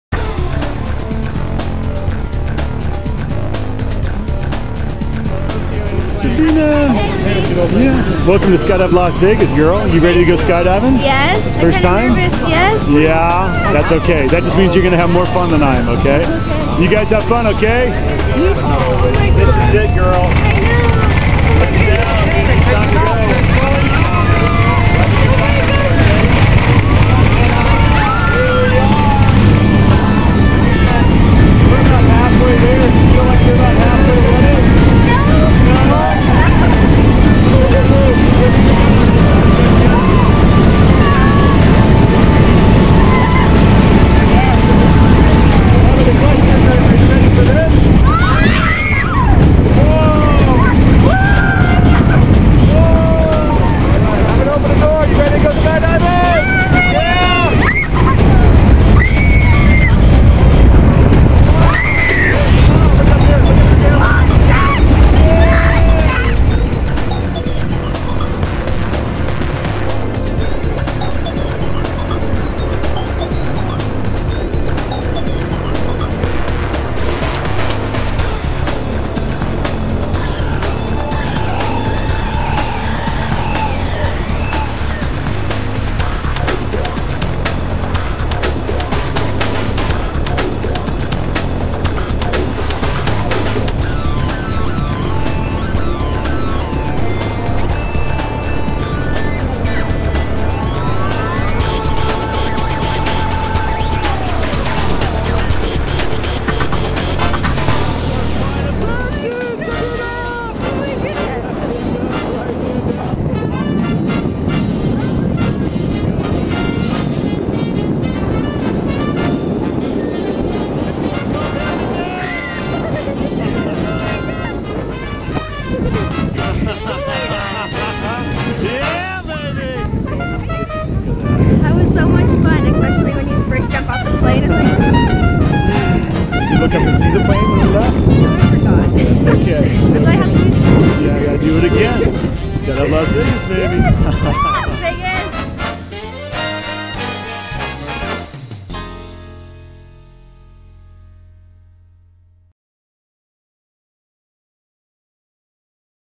SLV Daily Video Mar 31, 2012 - Skydive Las Vegas
Sabina having a blast on her first jump! Great job!